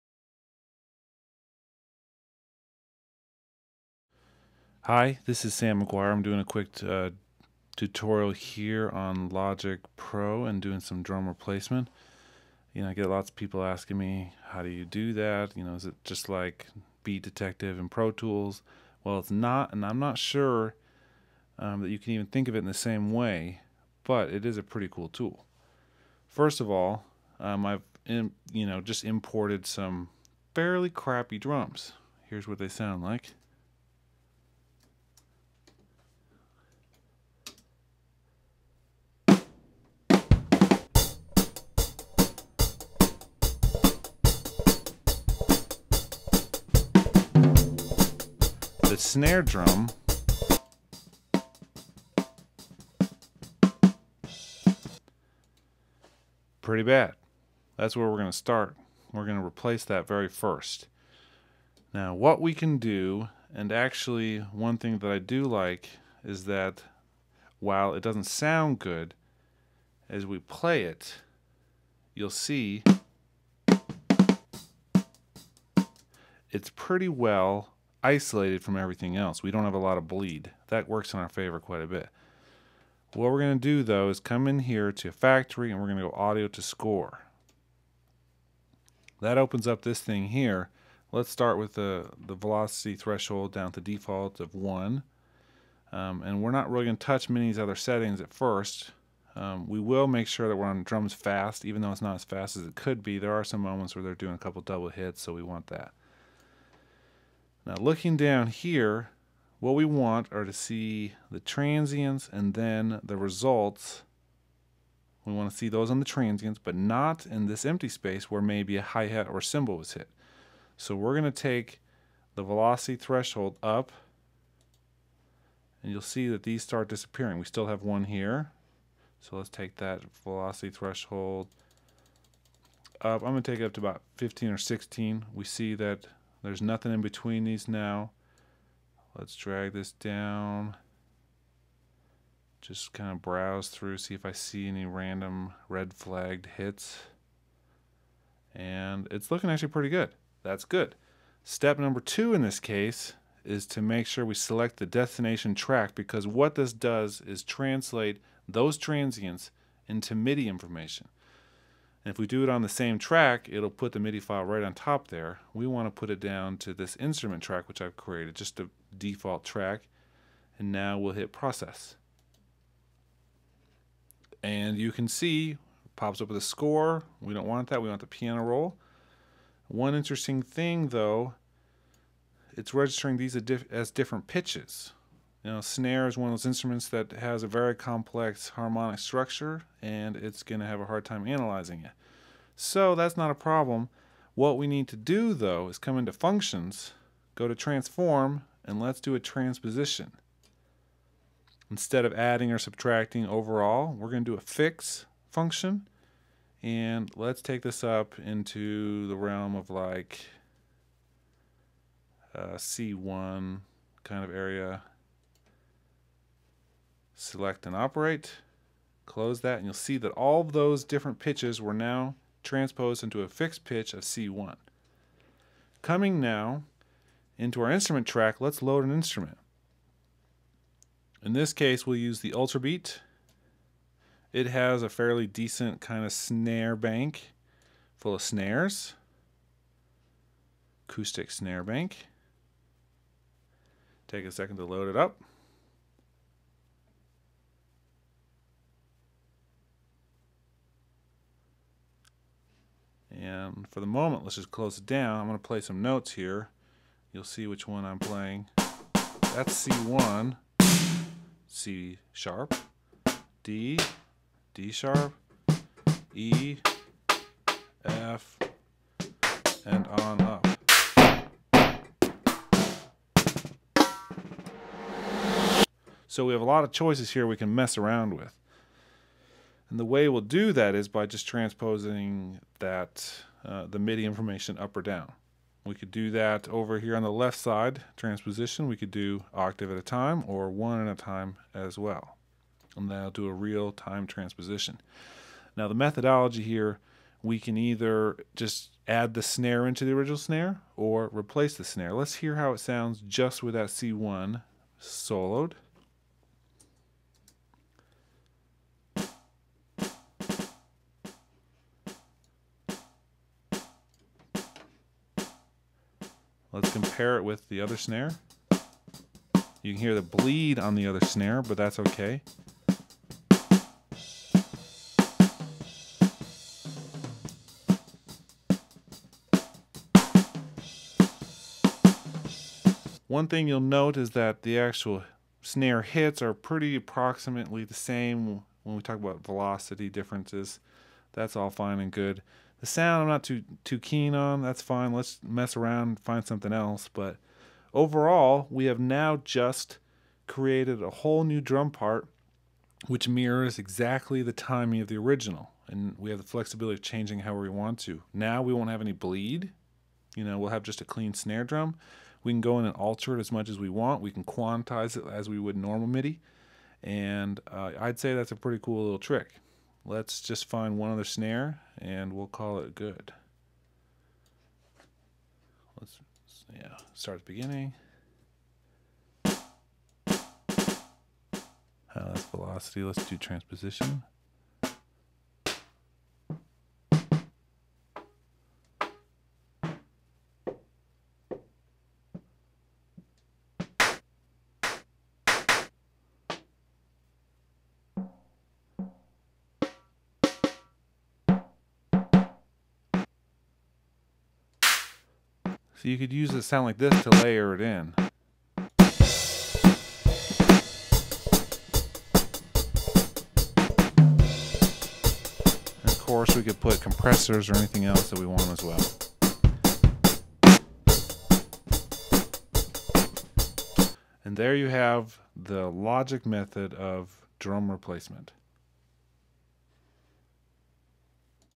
Logic 8 - Drum replacement tool
A short look at the audio to score tool in Logic 8.